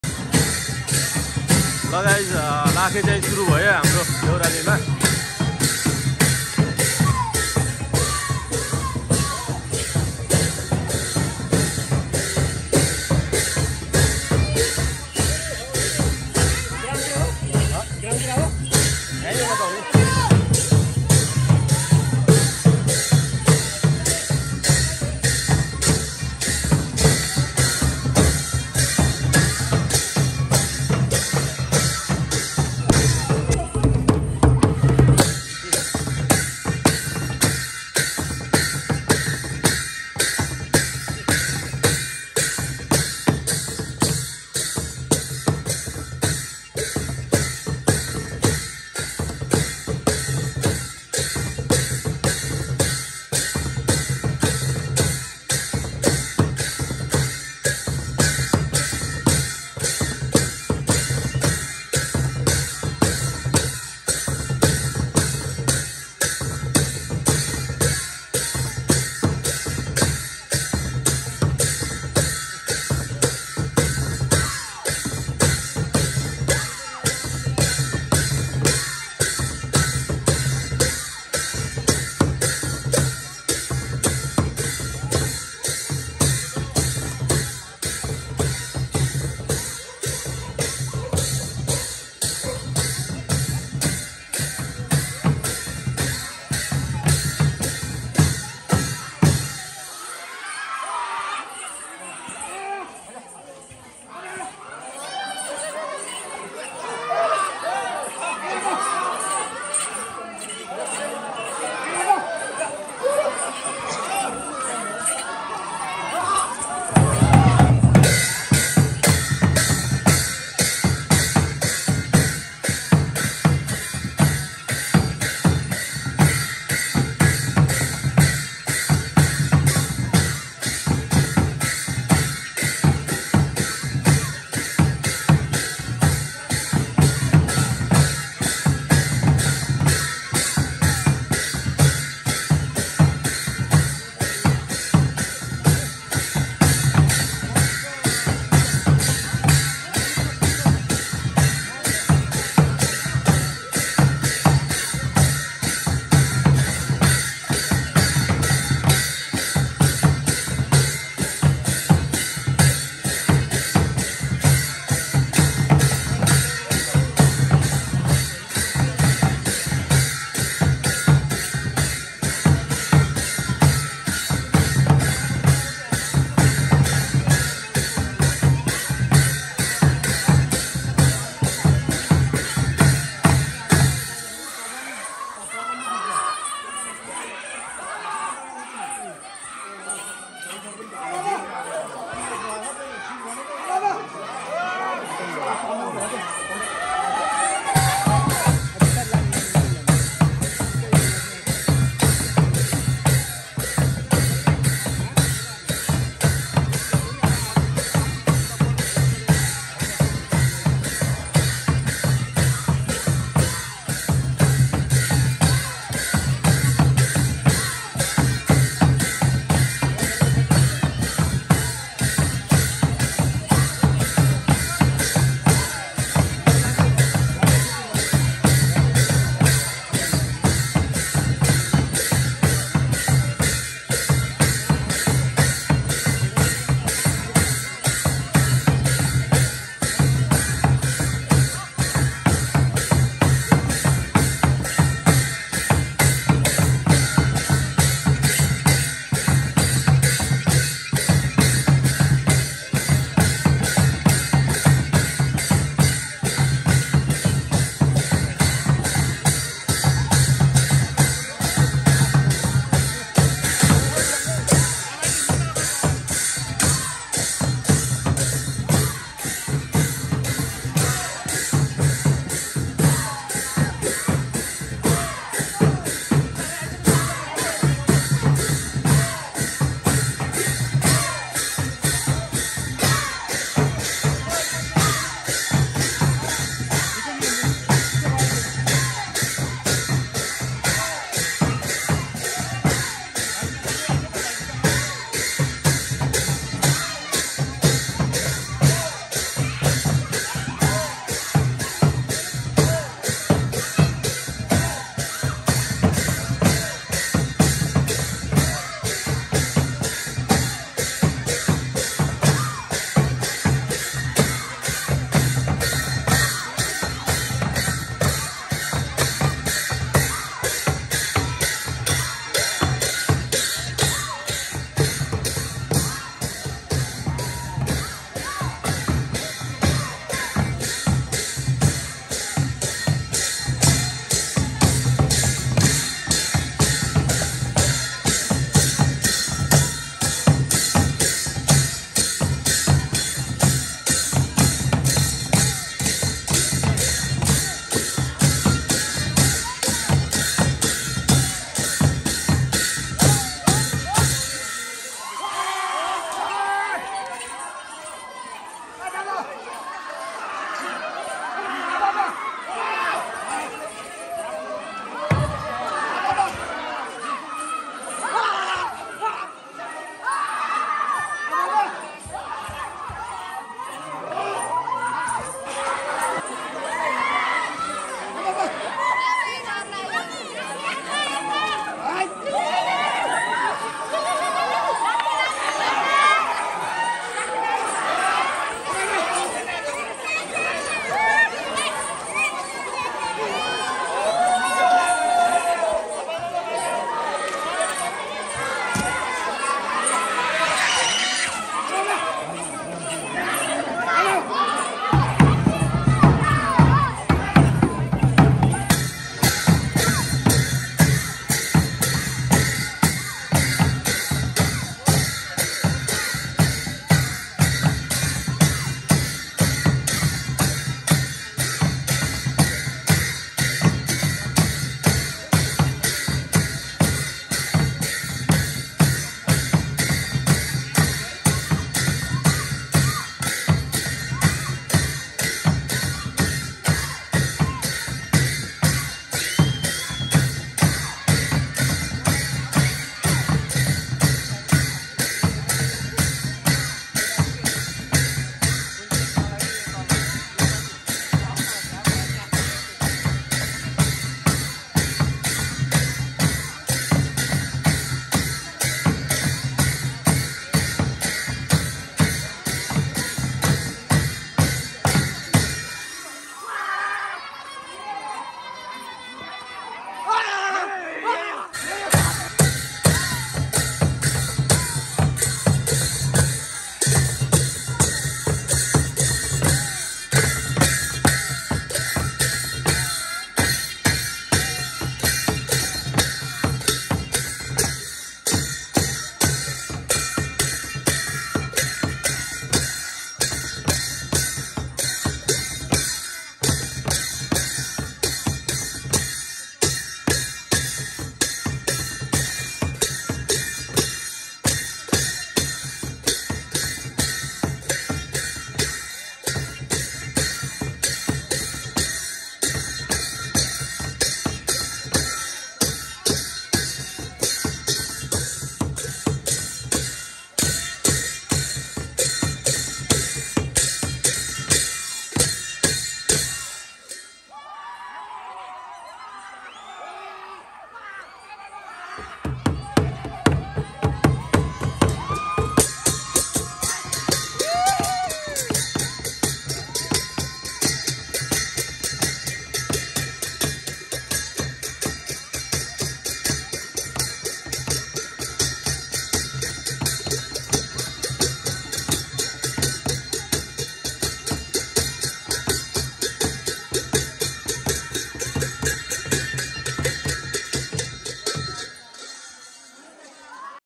यो बर्षको पहिलो लाखे नाच - Lakhe Nach 2081
रुपा देउराली नेवा खलको यो बर्षको पहिलो लाखे नाच हो | गठेमंगलको दिन नाचिएको यो लाख कस्तो लाग्यो कमेन्ट गर्नु होला !